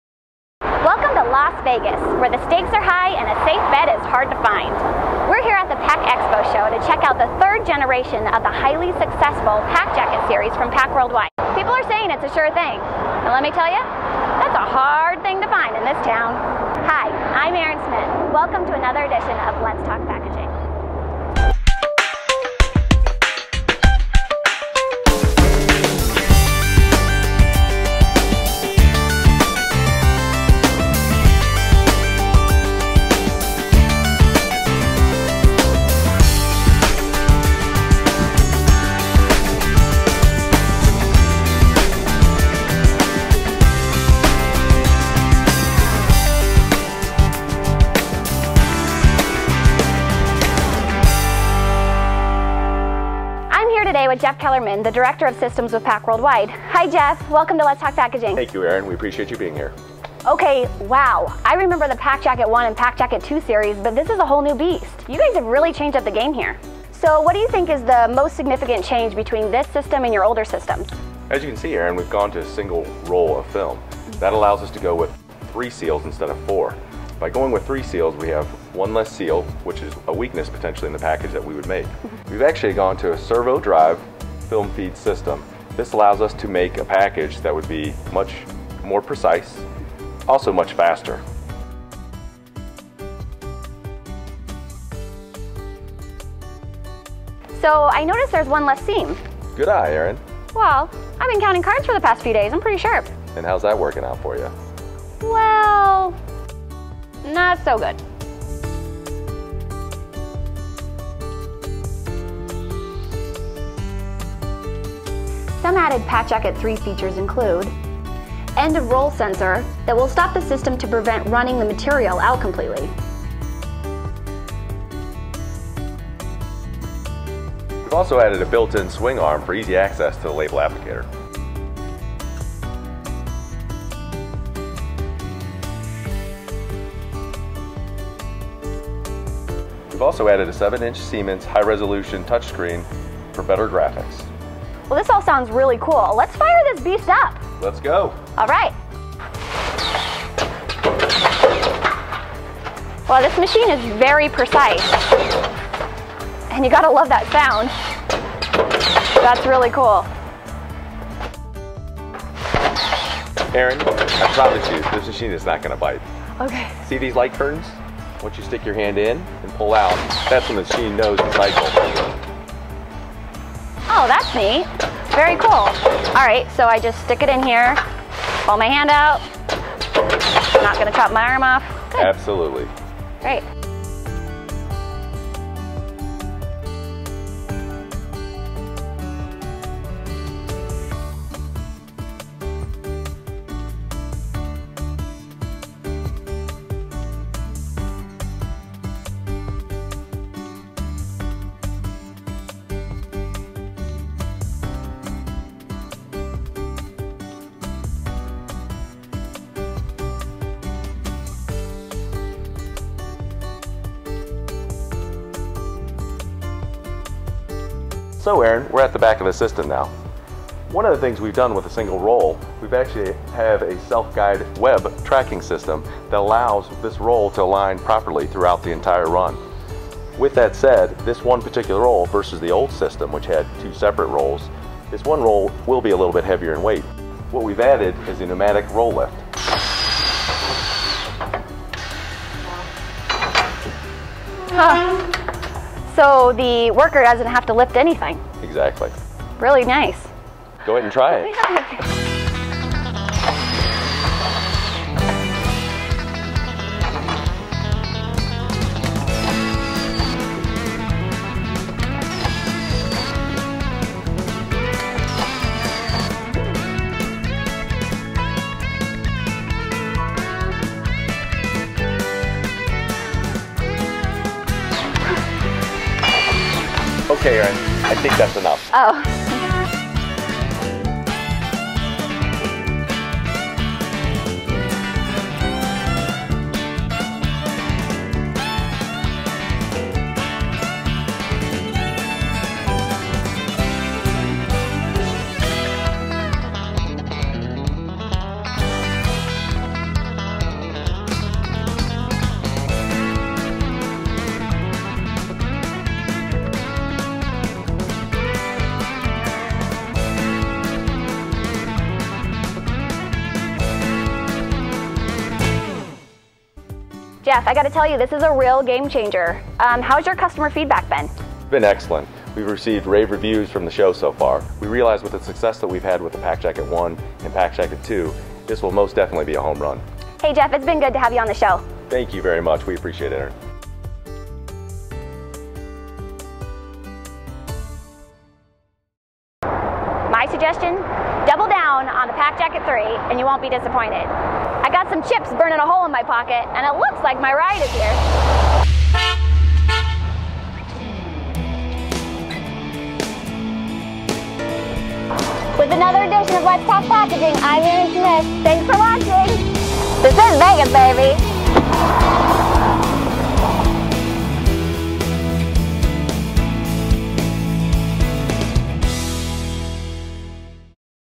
PACjacket Automated Packaging Machine
See the latest installment of the PACjacket automated packaging machine series from PAC Worldwide.  This video shows the innovative improvements PAC has made to the PACjacket machine, which provides customers with precise, fast, automated, packaging solution.

PACjacket
Quality, precision, and speed were what we had in mind for our customers when we developed the PACjacket automated solution. Our new single web design produces a high quality seal that offers greater security to its contents by reducing the number of heat seals necessary to produce a finished package off the system. The multitude of enhancements designed into this new PACjacket have increased our throughput speeds by as much as an astounding 25% when comparing to previous generations of the system.

On an average the PACjacket machine produces 15-20 bags per minute using protective bubble film or flat poly film.* By comparison, a person hand-loading and sealing kraft bubble mailers puts out an average of 3 bags per minute.* Increasing fulfillment efficiency by 400 to 500%!

*Output Varies

Added Features:
• 7" Full Color Touch Screen
• Single Web System
• Right Sizing Fiber Optic Eye Sensor
• Dual Sensor Safety System
• End of Roll Sensor
• No Heavy Lifting with Pneumatic Roll Lift
Specifications:
• Electrical: 220V, 20 AMP
• Air: 90 PSI
Capable of running:
• Standard 7/64" bubble film
• 1/2" bubble film
• Flat poly film